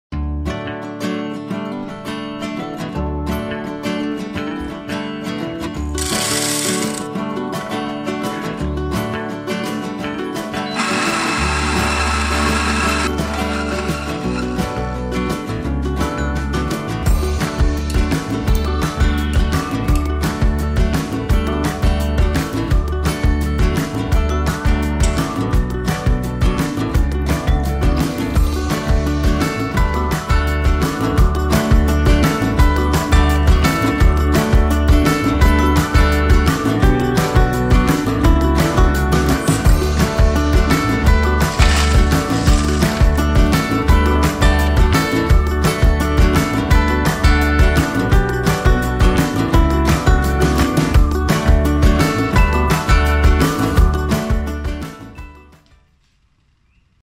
Espresso Soda - EP.24 - Indigo Child Daily Coffee
Our daily coffee at Indigo Child Coffee.

Line: @indigochildcoffee

Recipe
1.  2 shots of Espresso (18g of House Blend Coffee).
2. 160ml of soda water.
3. Ice.
4. Syrup of your choice.